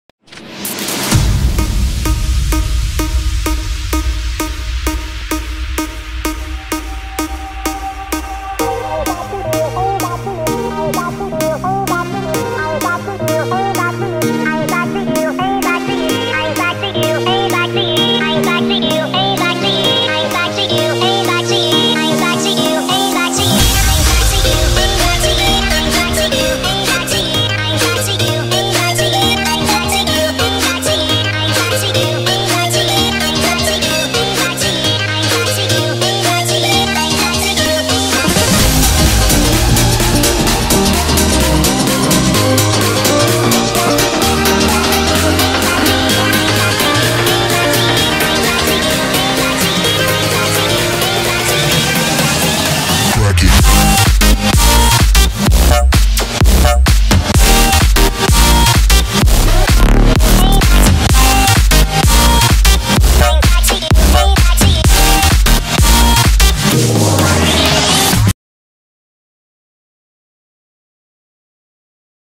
join fly light air! (this was a tets flight for the flight later on)
flight later at new airprot at 20 (8 o'clock) GMT see you there join me! sorry for no upload i was very busy